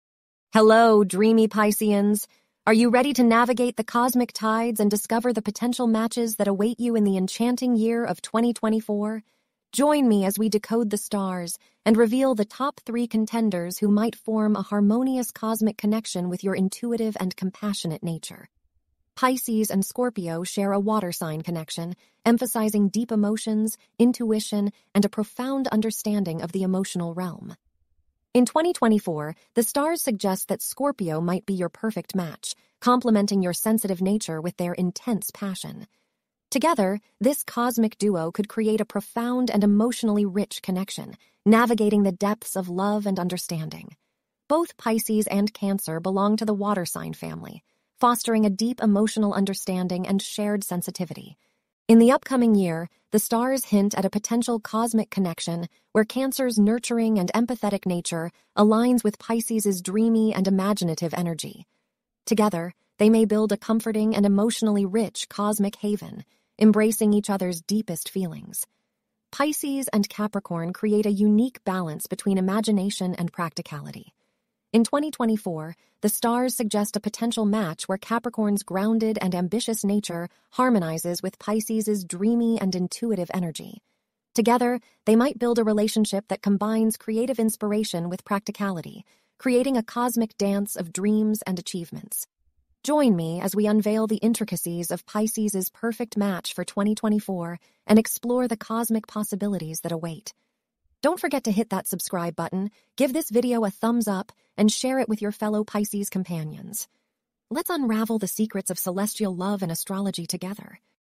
Who is Pisces's true soulmate? [ 2024 ]
Hello, dreamy Pisceans! Are you ready to navigate the cosmic tides and discover the potential matches that await you in the enchanting year of 2024? Join me as we decode the stars and reveal the top three contenders who might form a harmonious cosmic connection with your intuitive and compassionate nature.

Pisces and Scorpio share a water sign connection, emphasizing deep emotions, intuition, and a profound understanding of the emotional realm. In 2024, the stars suggest that Scorpio might be your perfect match, complementing your sensitive nature with their intense passion. Together, this cosmic duo could create a profound and emotionally rich connection, navigating the depths of love and understanding.

Both Pisces and Cancer belong to the water sign family, fostering a deep emotional understanding and shared sensitivity. In the upcoming year, the stars hint at a potential cosmic connection where Cancer's nurturing and empathetic nature aligns with Pisces's dreamy and imaginative energy. Together, they may build a comforting and emotionally rich cosmic haven, embracing each other's deepest feelings.

Pisces and Capricorn create a unique balance between imagination and practicality. In 2024, the stars suggest a potential match where Capricorn's grounded and ambitious nature harmonizes with Pisces's dreamy and intuitive energy. Together, they might build a relationship that combines creative inspiration with practicality, creating a cosmic dance of dreams and achievements.

Join me as we unveil the intricacies of Pisces's perfect match for 2024 and explore the cosmic possibilities that await. Let's unravel the secrets of celestial love and astrology together!